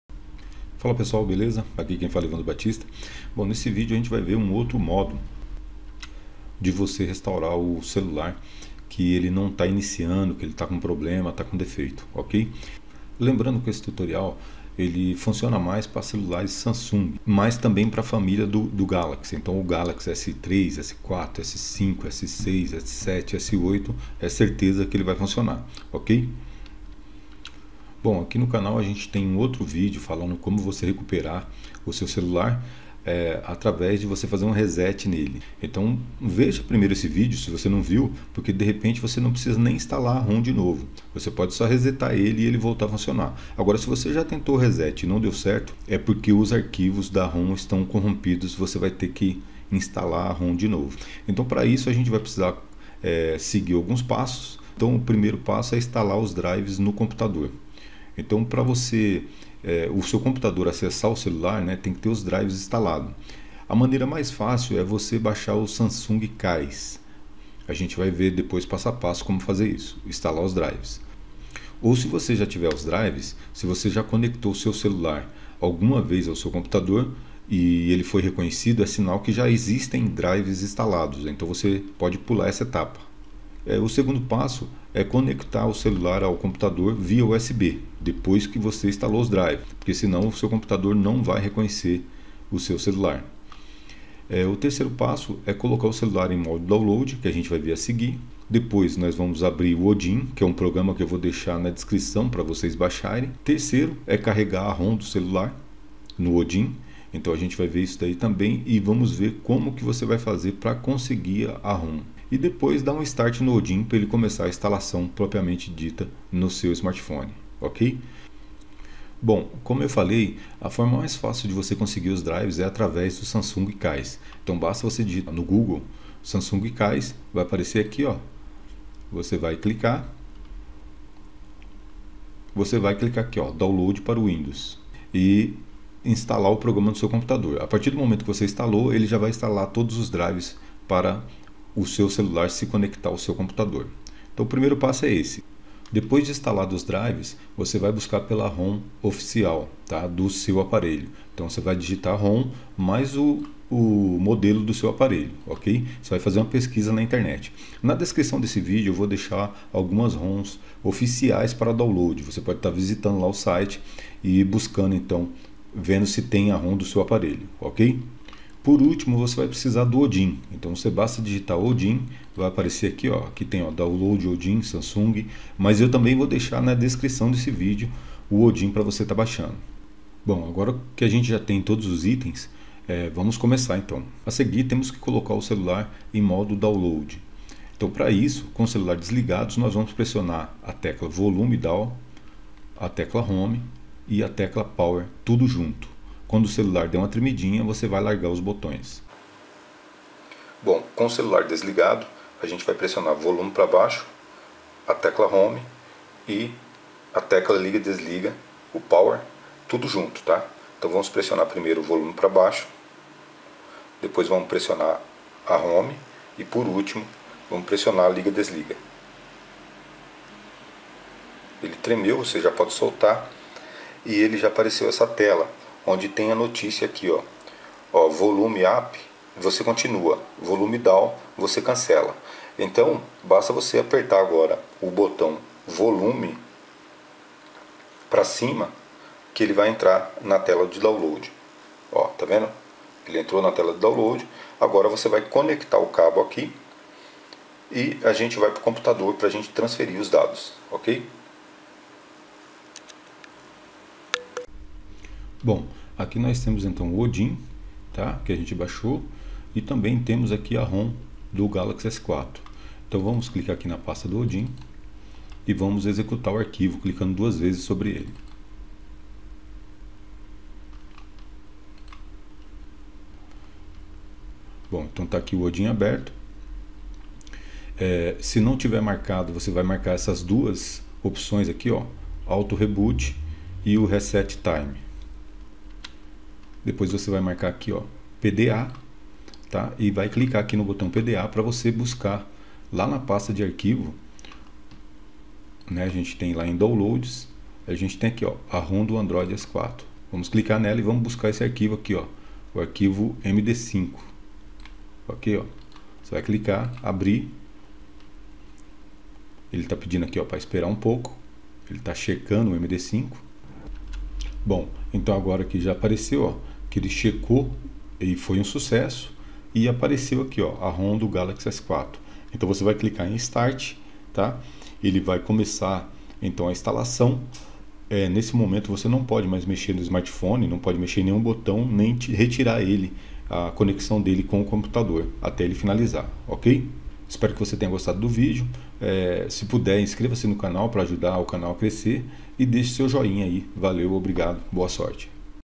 Como recuperar celular Android brickado. Reinstalar Rom original
Como reinstalar a Rom original do seu celular Android